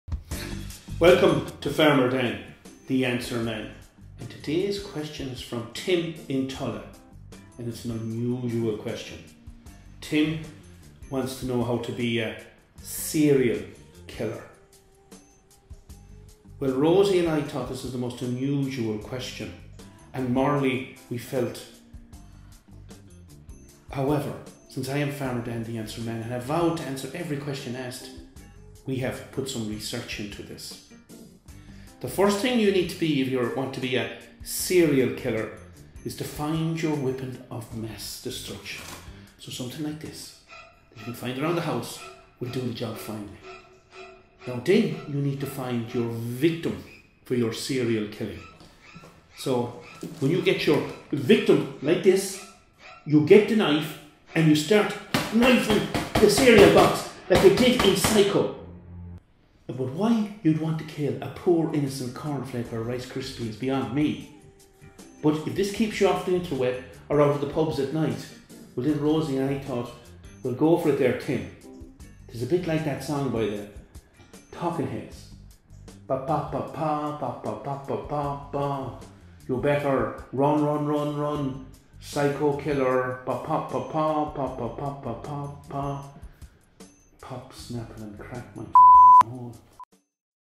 Farmer Dan the Answer-man on 'How to be a Serial Killer?'
Farmer Dan the Answer-man answers a question from Tim in Tulla on 'How to be a Serial Killer?'
Both Farmer Dan the Answer Man and Rosie dog found this question disturbing yet Farmer Dan has vowed to answer all questions.